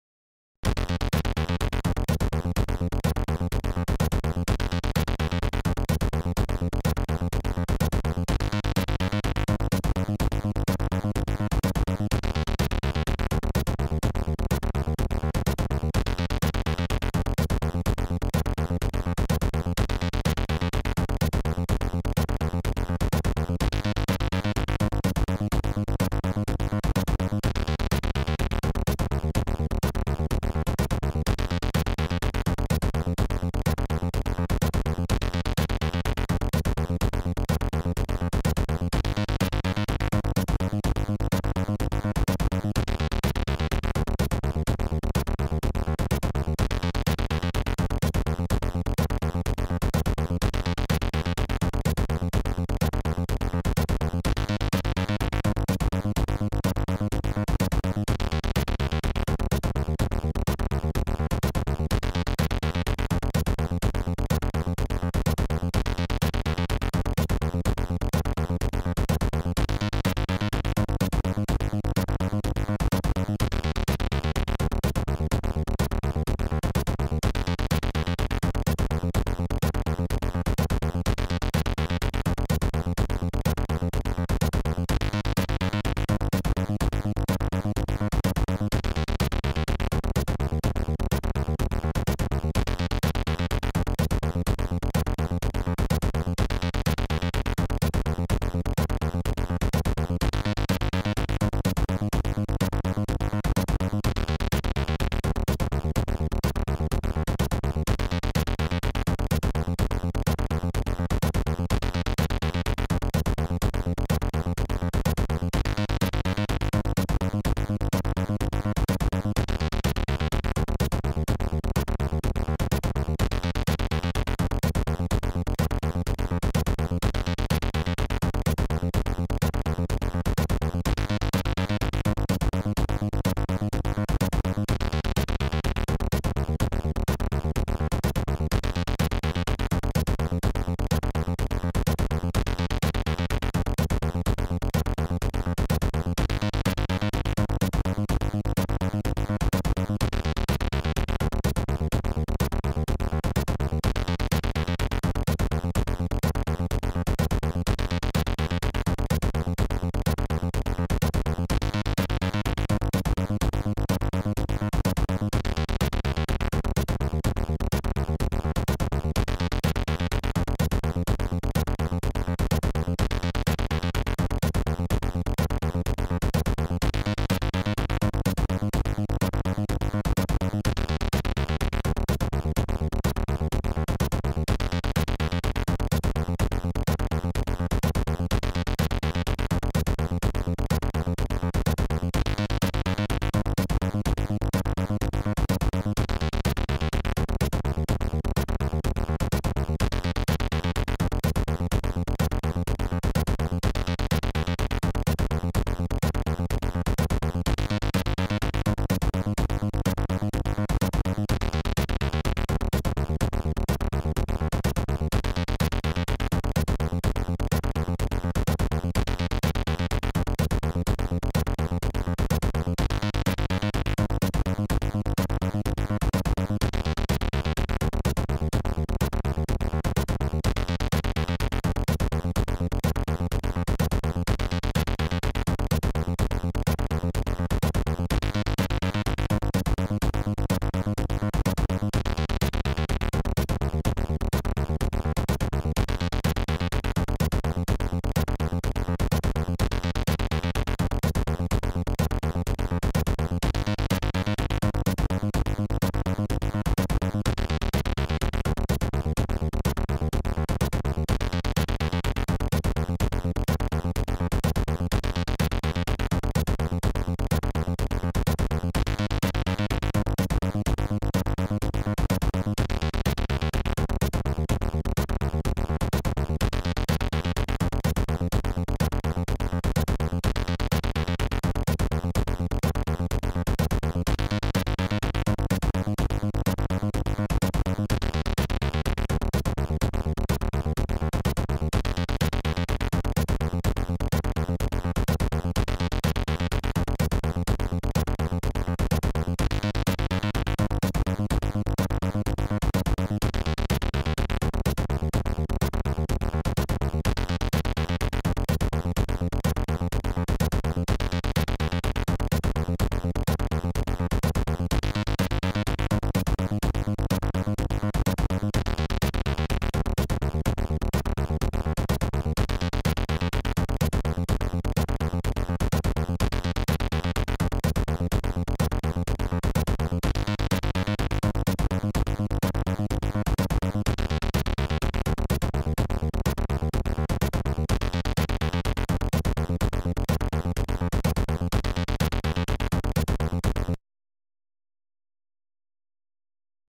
Big Pixel Intro for C64
My first attempt at creating something intro-like. It is a rather large for an intro, almost 16k, but I really wanted to see what I can do with 80x50 screen and softsprite routines that I made. Music is made by Hasse Axəlsson-Svala for CRX 2020, I used it with his permission as I have zero music talent. All was coded using the fantastic TRSE.